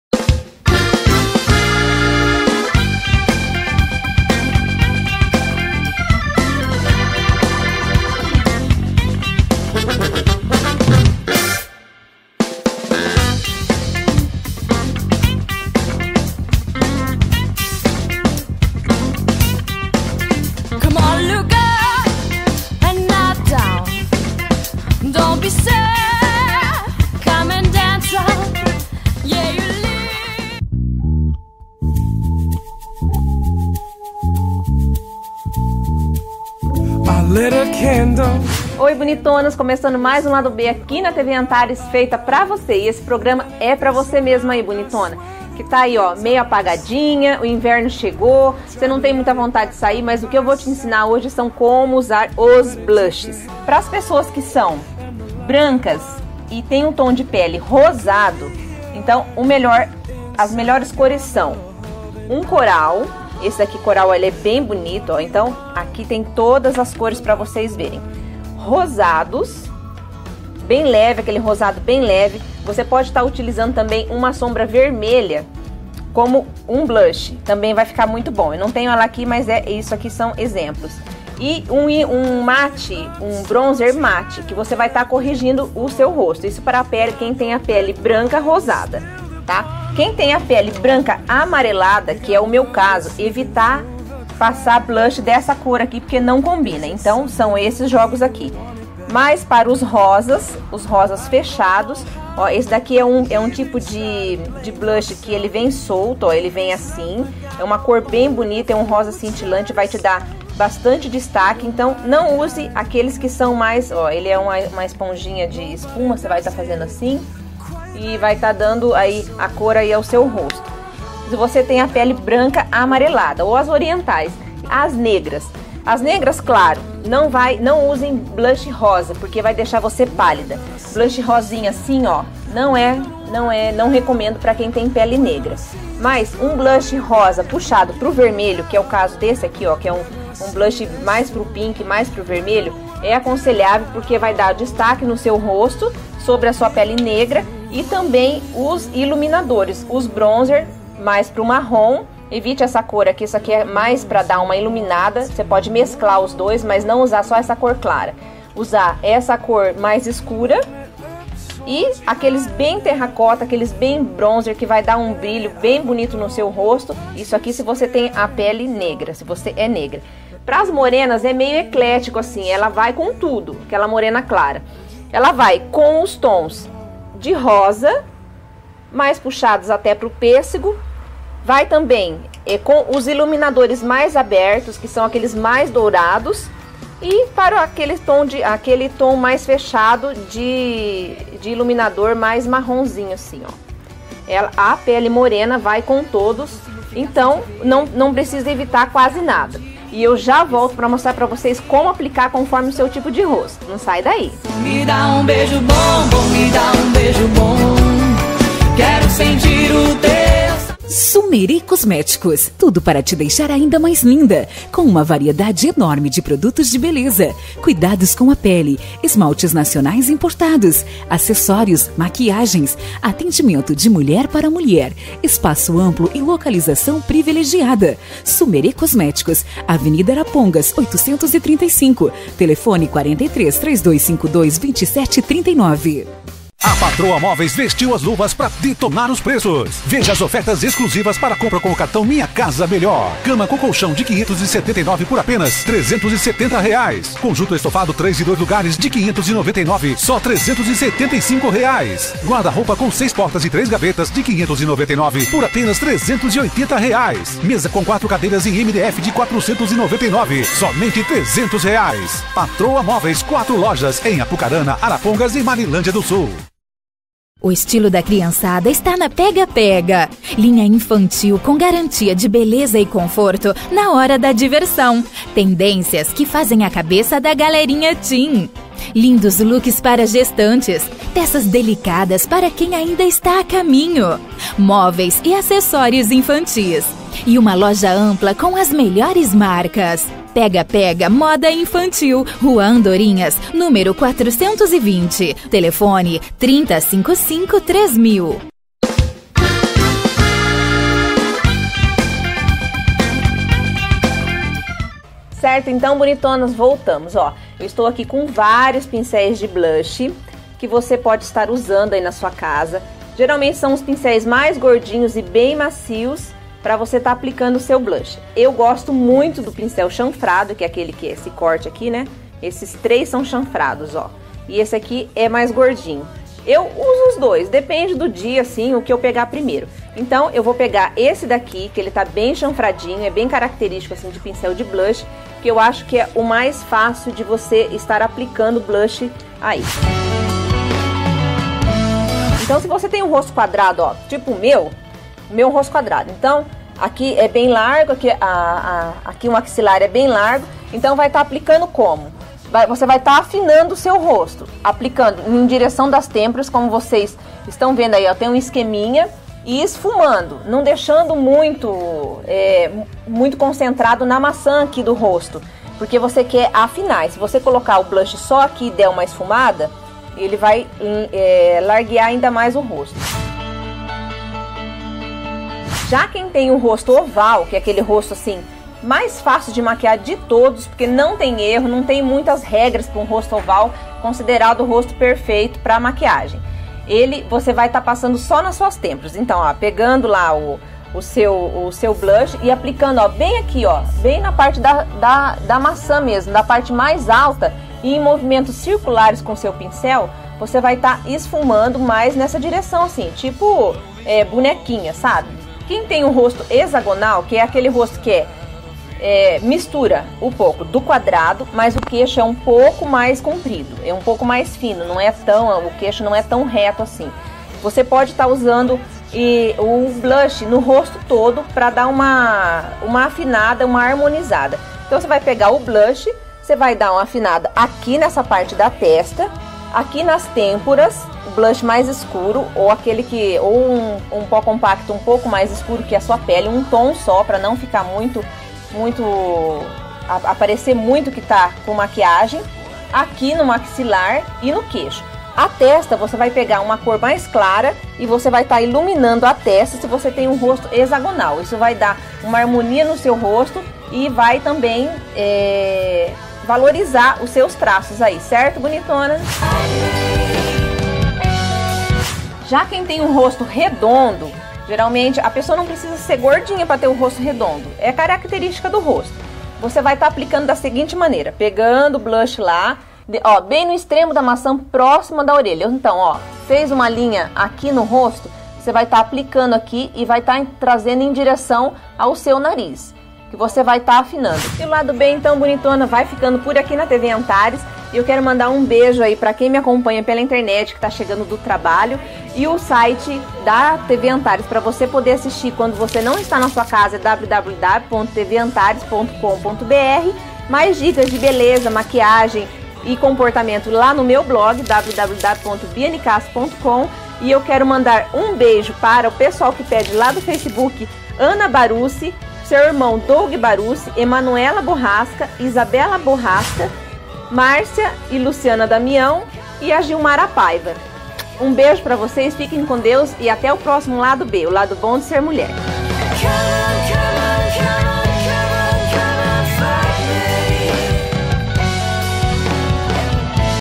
LADO B - Blush e como aplicá-lo conforme seu formato de rosto
Bia Nicastro mosra quais as cores de blushes para cada tom de pele e como aplicá-lo levando em conta seu formato de rosto.